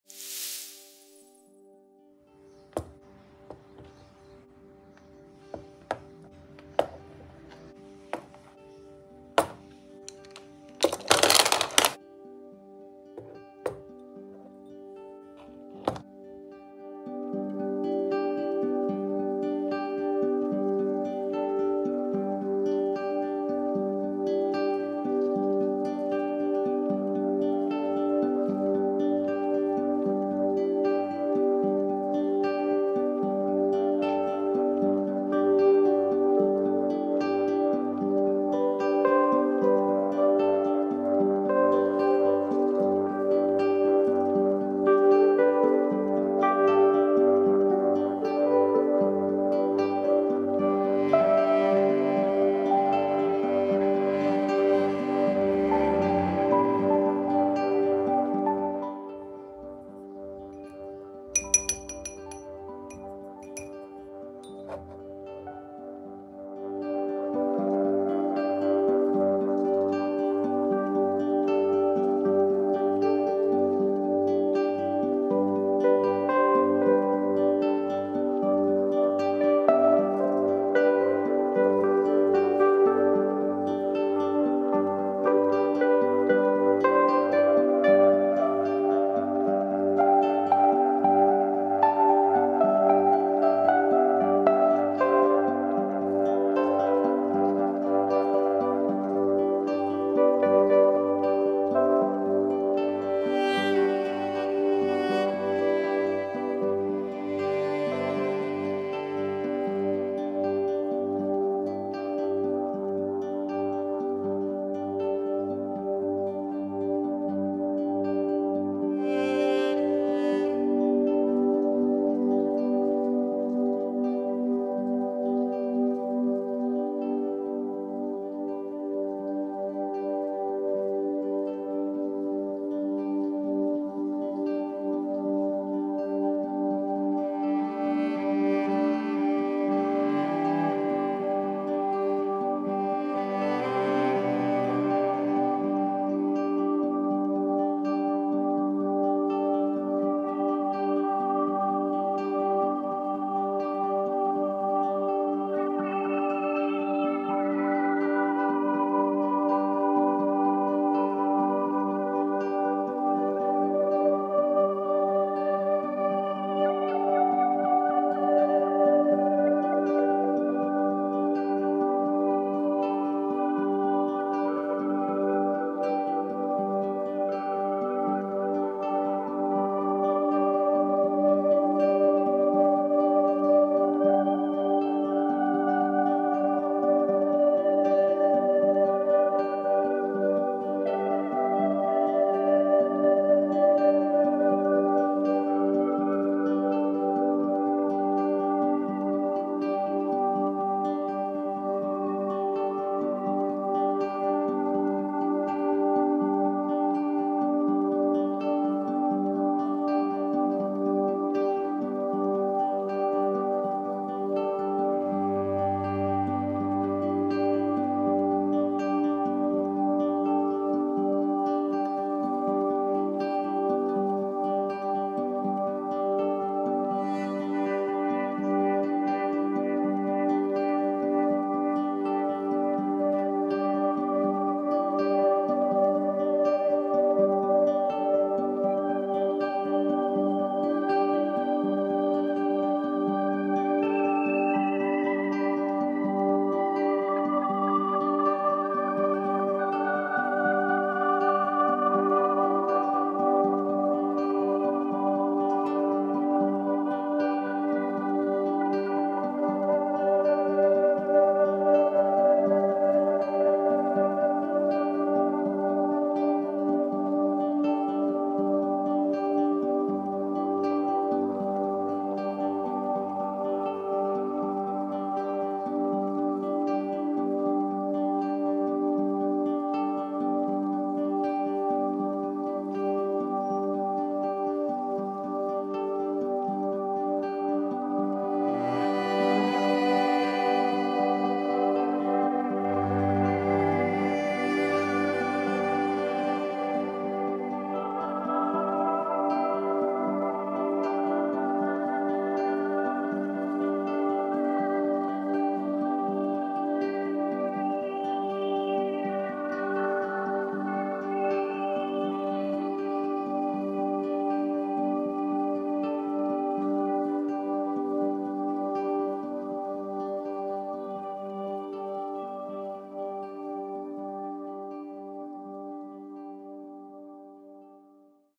How to acrylic painting on stone | a cup of coffee in nature | Step by Step
I love acrylic painting on stones ✨
What about you?✨
Comment your opinion about this painting✨